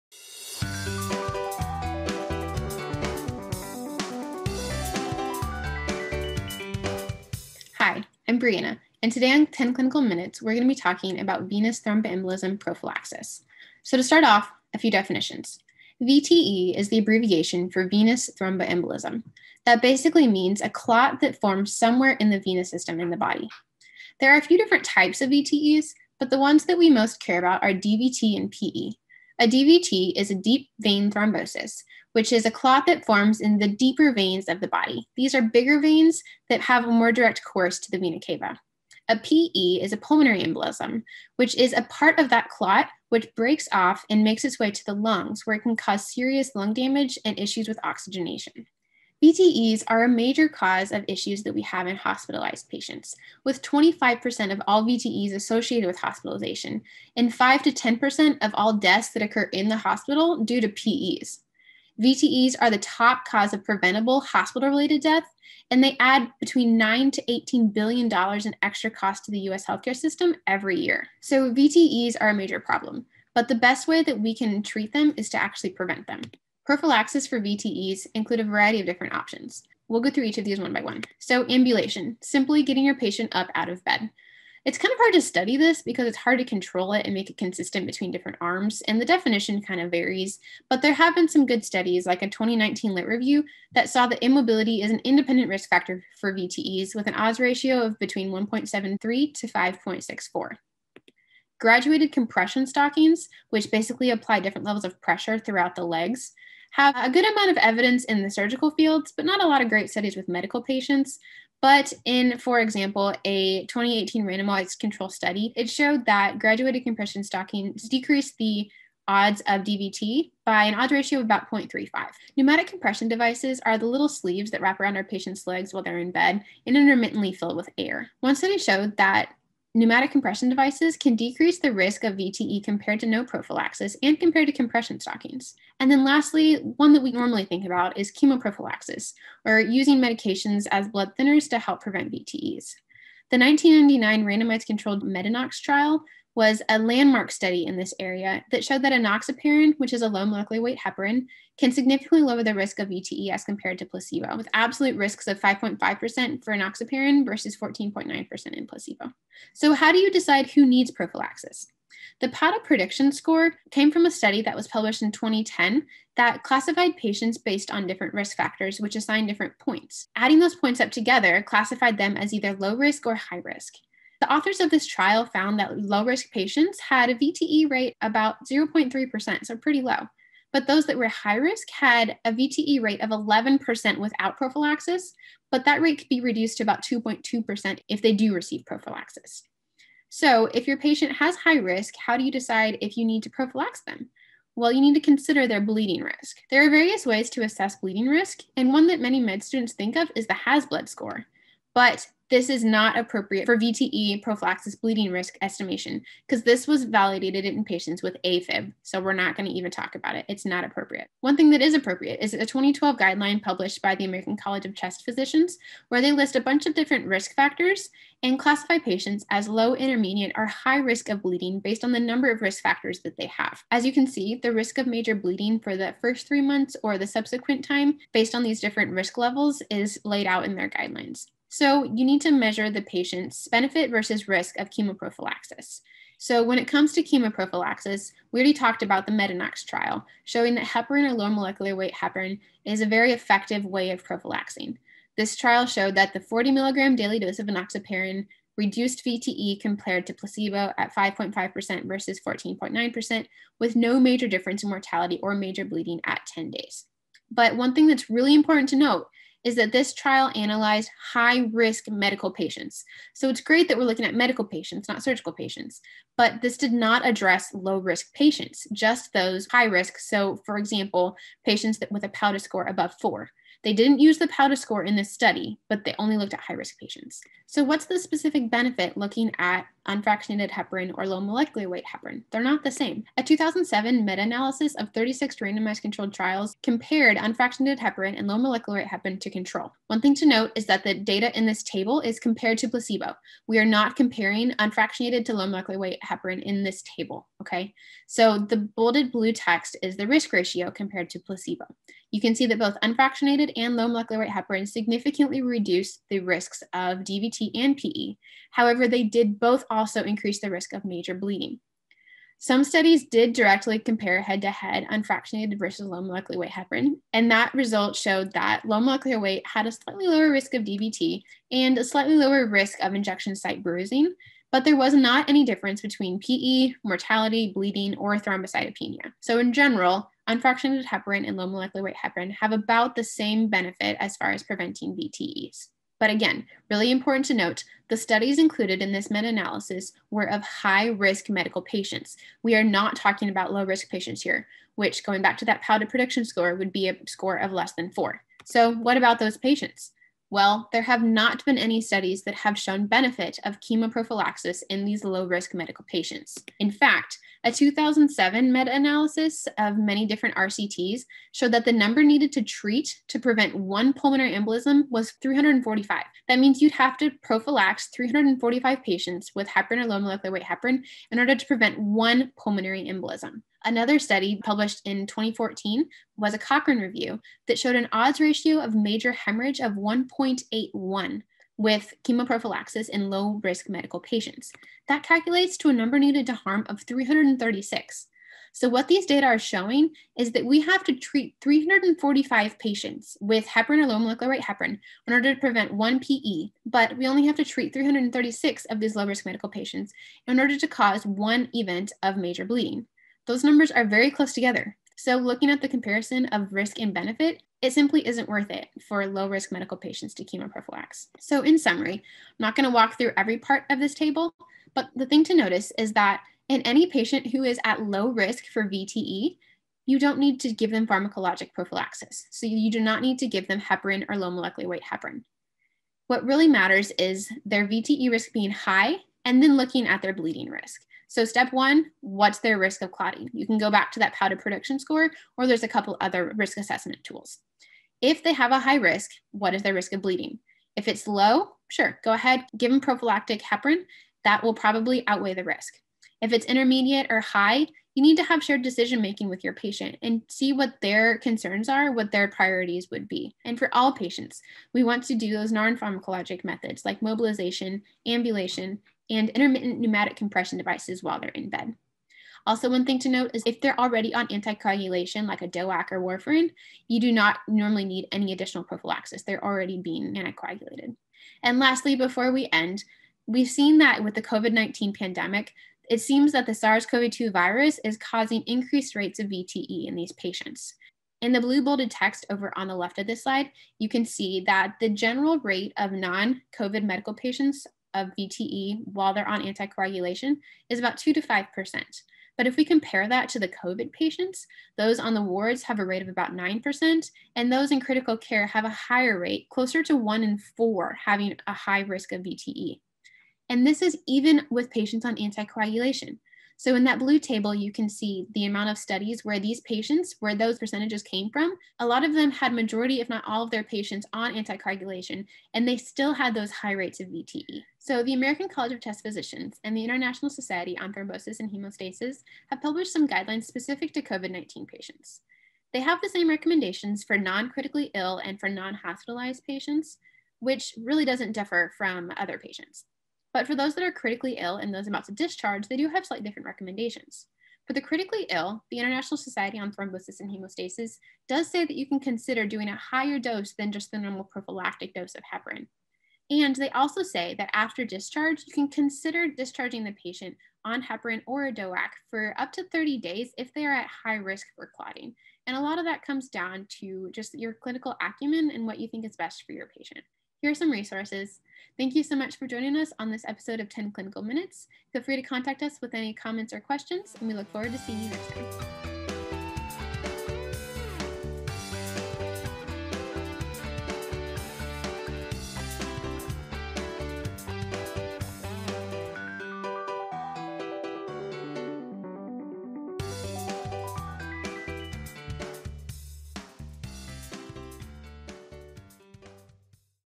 10 Clinical Minutes: VTE Prophylaxis
Venous thromboembolism is a source of considerable morbidity & mortality in hospitalized medical patients. But, there's a fine line between preventing clots and causing bleeding. In this episode we discuss non-pharmacologic & pharmacologic prophylaxis options, how to weigh the risk of VTE against the risk of bleeding, and which patients to consider for which type of prophylaxis.

Time stamps:
0:00 Intro
1:16 Prophylaxis options
2:39 Risk scoring: VTE
3:16 Risk scoring: Bleeding
3:59 Chemoprophylaxis
5:48 Low-risk patients

6:50 Summary
7:50 COVID-19
9:20 Outro

Written & presented by Brianna Coogle, MS4
Reviewed by Abebe Abebe, MD
Edited by William Huynh, MS3